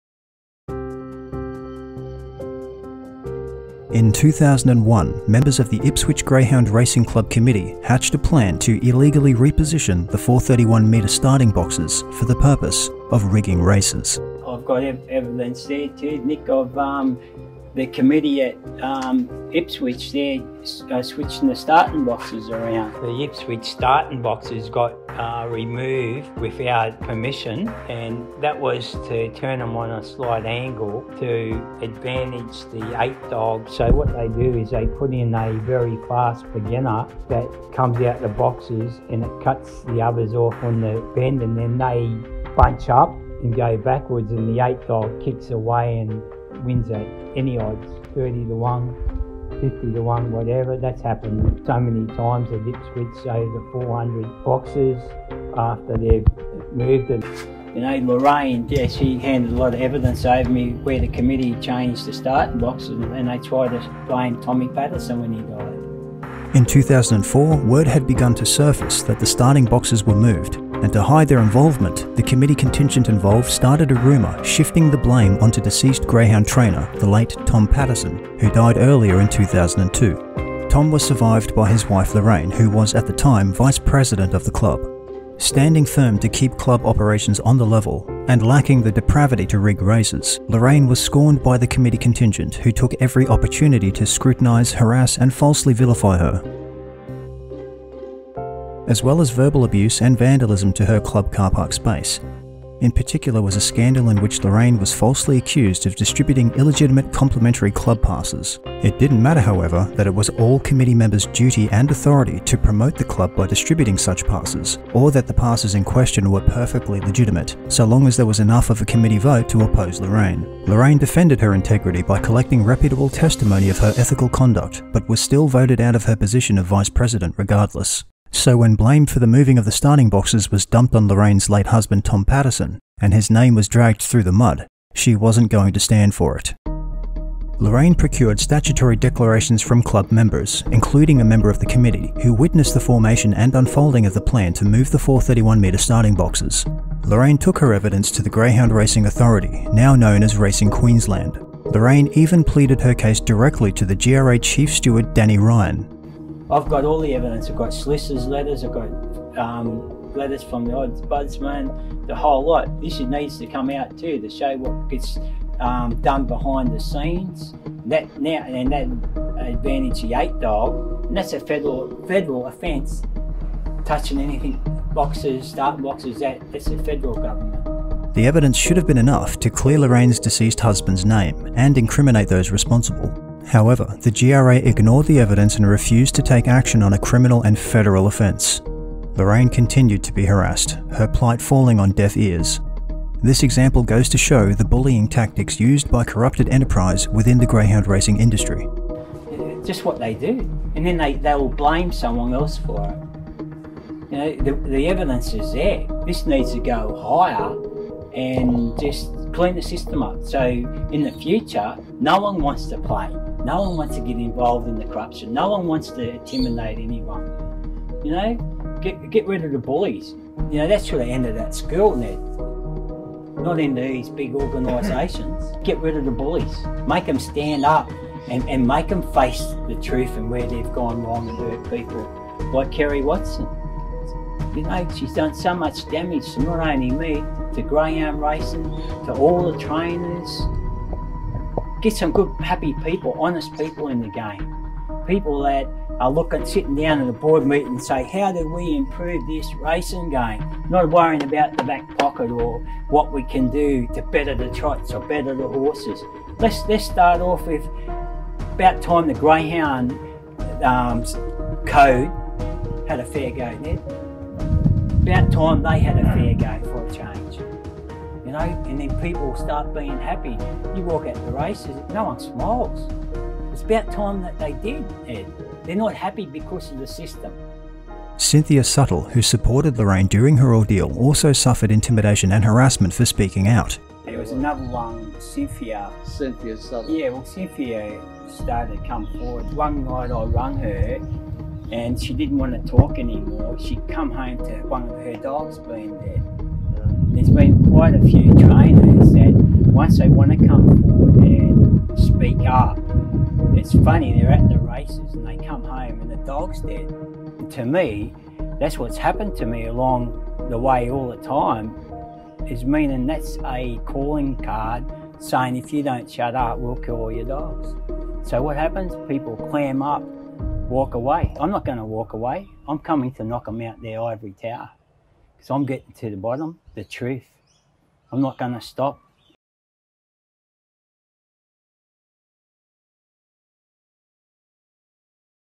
Ipswich Greyhound Race Rigging
Ipswich Greyhound Racing Club committee members Merv Page, Arnold Dull, Rob Whittington, and Sam Brady, along with IGRC employee Noel Weber, conspire to move the 431 metre start boxes in a race rigging plot. But when it begins to surface that the boxes were moved the blame is pinned on deceased trainer Tom Paterson.

Tom's wife Larraine defends his name and gets bullied out of her position of Vice President of the club. Larraine takes the matter to the Greyhound Racing Authority (now Racing Queensland) but instead of investigate a federal offence, they refuse to look at Larraine's evidence and sit on their hands instead.

This story exemplifies the classic bullying and intimidation tactics corrupted enterprise within the Greyhound Racing Industry will use to protect their criminal profiteering.

Join as we continue to blow the lid on Grey Hound Racing Industry corruption.